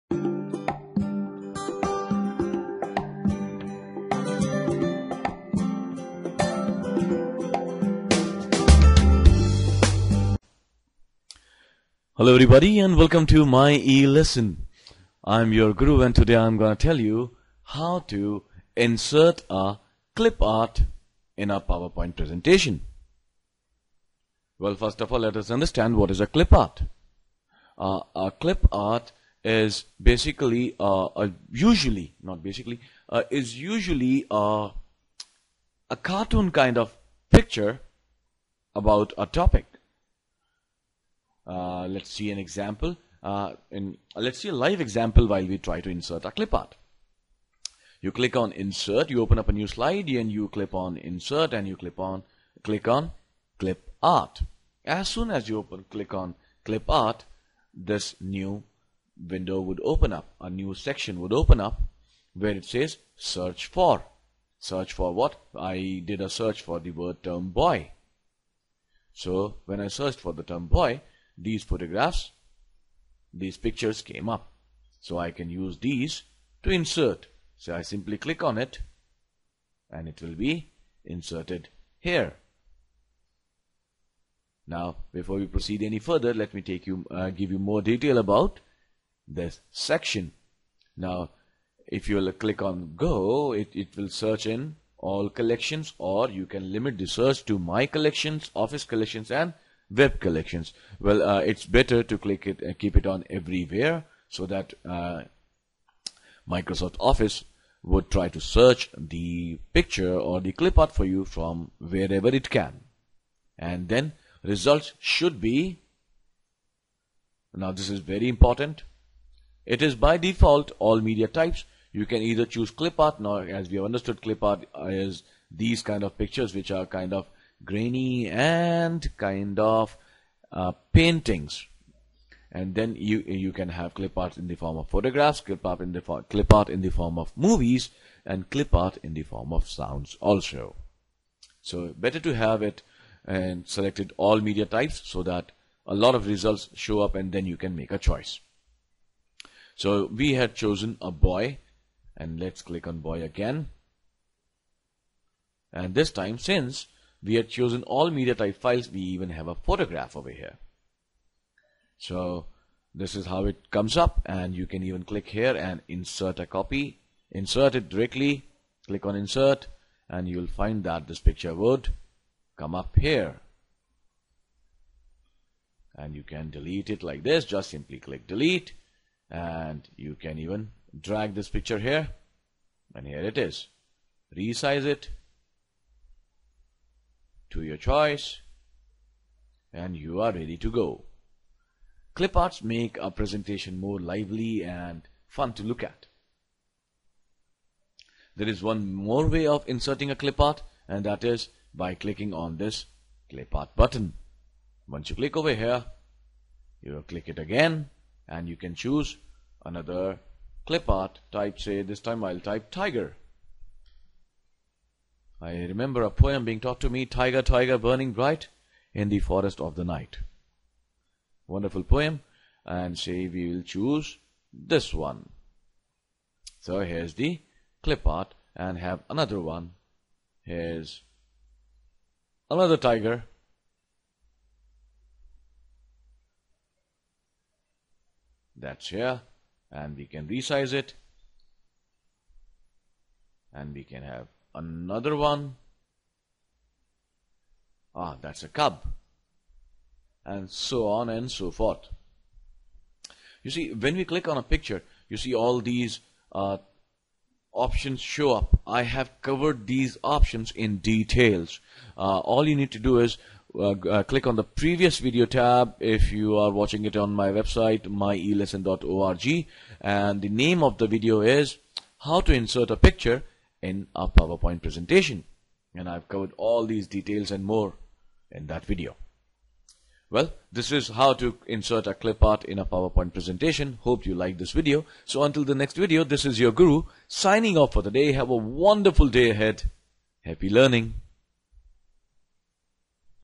Insert Clip Art In PowerPoint Presentation
Insert a ClipArt image in Microsoft PowerPoint Presentation.
Add a clipart image in PowerPoint presentation to make your point stick and its really super easy to put a clipart in ppt as shown in this video.

Add online pictures or clip art to your file

Applies To: Excel 2016 Word 2016 Outlook 2016 PowerPoint 2016 OneNote 2016 More...
You can add clip art or online pictures to a file in one of two ways, depending on which version of Office you're using.

Insert - ClipArt (2007, 2010):

How to add clip art in Office 2010 and 2007 apps
Insert - Online Pictures (2013, 2016):

Screenshot of how to add online pictures into Office apps.
Office 2007 and 2010
In Office 2007 and 2010, you can go directly to Bing and search for images. You'll want to be sure that you have the rights to use the images you select, and you can do that by setting easy-to-use licensing filters. Here's how:

Go to Bing Image Search.

In the Search box, enter the type of image you're looking for.

Query entered into Bing Image Search box

When the image results appear, click or tap the License drop-down menu, and pick the licensing option you want. (If you don't see the License filter, make sure you're using a supported browser.)

License drop-down set to Free to modify, share, and use commercially.

NOTES:

Supported browsers include Internet Explorer 9 or later, Mozilla Firefox (any version), Apple Safari (any version), Google Chrome (any version), and Amazon Kindle Fire (any version).

If you want to narrow results to clip art (cartoon-like images), add "clip art" to the end of your search term or change Bing's Type filter to Clipart.

Click the image if you want to see it full-size.

Now, make a local copy. For example, in Internet Explorer, right-click the image, and choose Save picture as. In Chrome, right-click the image, and choose Save image as.

In your Office app, go to Insert - Picture and find the image you just saved from Bing.

To better understand the licensing options, see Filter images by license type. Remember, you're responsible for respecting the rights of others' property, including copyright.

Office 2013 and 2016
In Office 2013 and 2016, you don't need to open your browser or leave your document to insert images.

Choose Insert - Online Pictures in any Office app.

Choose Bing Image Search, and type a search term in the Search box to see images from Bing.

If you choose All Images, your search results will expand to show all Bing images. You're responsible for respecting the rights of others' property, including copyright. To better understand the licensing options, see Filter images by license type.

NOTE: If you see an inappropriate image, move your mouse pointer over it, click the Report Image  Screenshot of the Report Image icon. icon that appears, and report the image as inappropriate.

Screenshot of the dialog box where you can add clip art in Office apps.

Select an image and choose Insert.